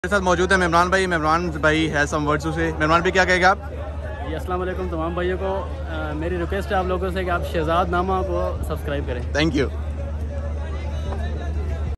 Mimran Khan has a word to Say | Shehzadnama
Mimran khan is a well know player of tape ball cricket. This clip is captured during final match of Markhor Champions League 2021